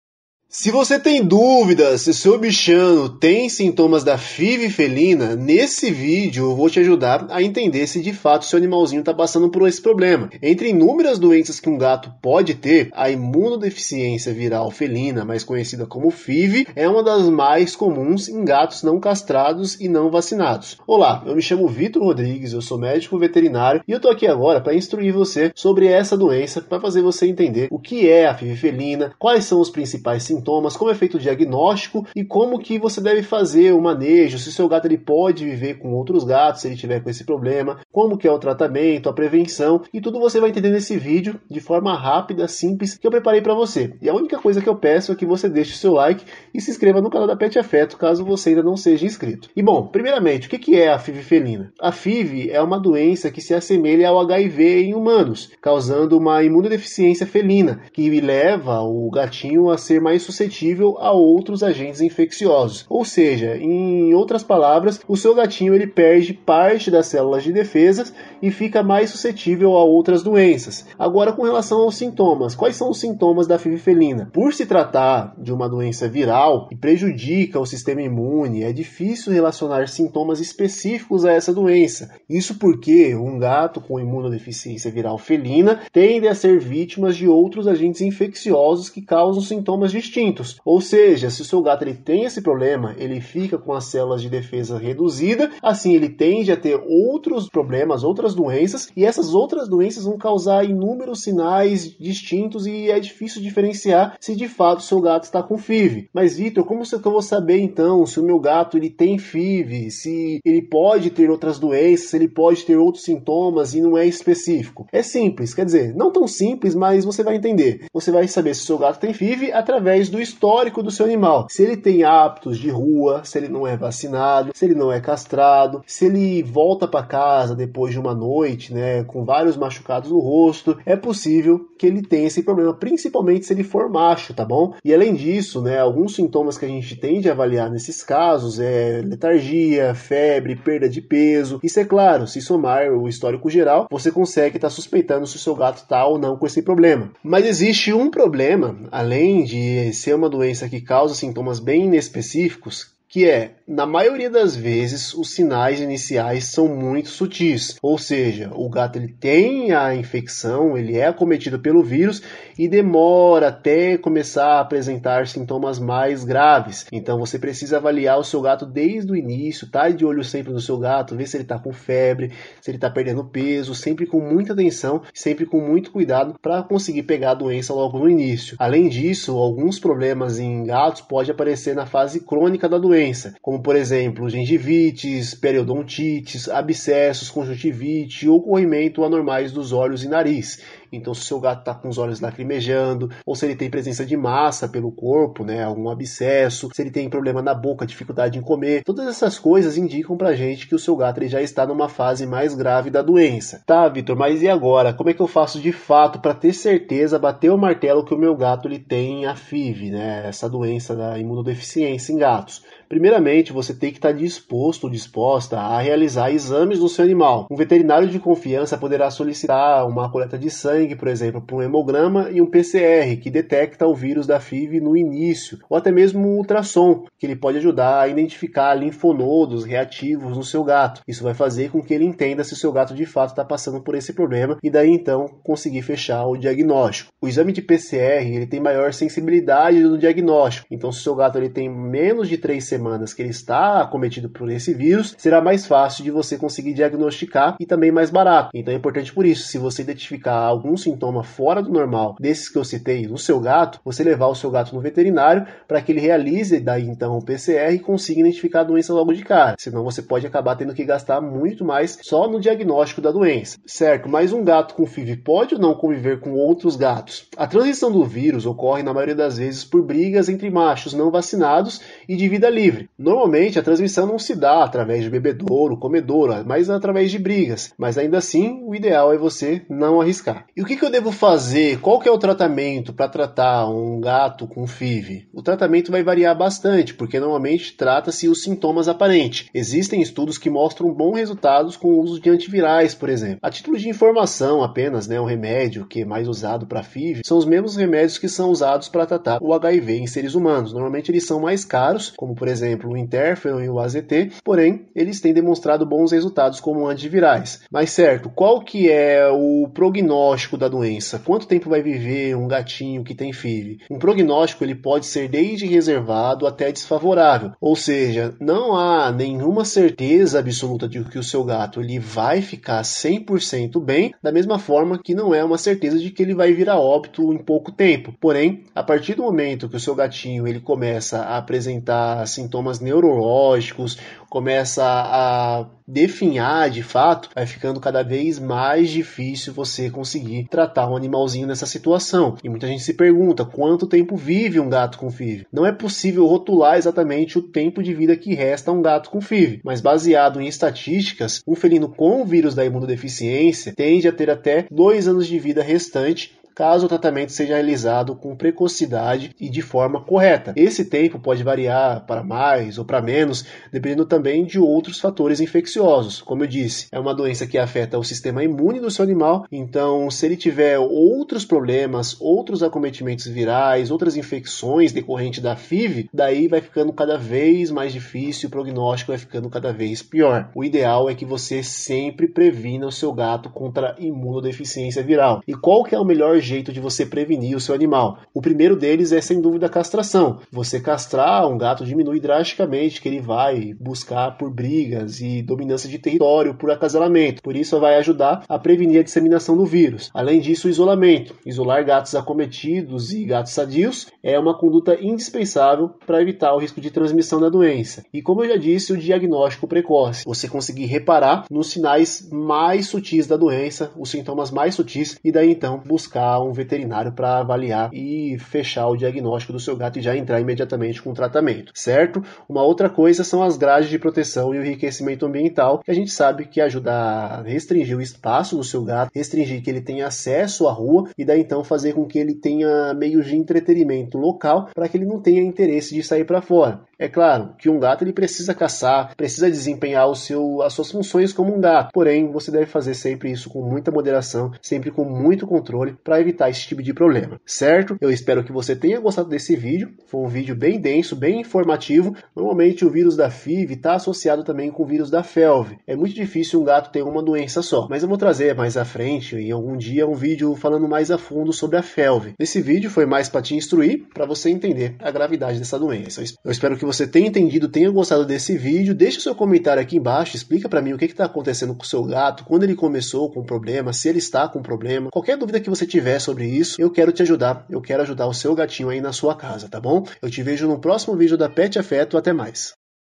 Sintomas da FIV - Quanto tempo vive um GATO com FIV.
O vírus da imunodeficiência felina, também conhecido com FIV, é semelhante a aids em humanos. Porque causa sinais muito semelhantes e o tratamento também é muito parecido tal como a expectativa de vida.

Os sintomas fiv felina são bem inespecíficos, isso porque o vírus diminui as células de defesa dos gatos deixando-o propenso a outras infecções.

Muita gente se pergunta se gato com fiv pode conviver com outros gatos, em casos assim o ideal é se atentar a transmissão do vírus, além de tomar outras medidas de prevenção.

Como dito o remédio para fiv felina é o mesmo usado para tratar a aids em humanos, com algumas particularidades, contudo o ideal é recorrer a um veterinário para que ele(a) prescreva medicações para tratar os sintomas da aids felina.

Quanto tempo vive um gato com fiv? Tudo depende da percepção da doença e da sua conduta frente ao problema, entenda tudo nesse vídeo.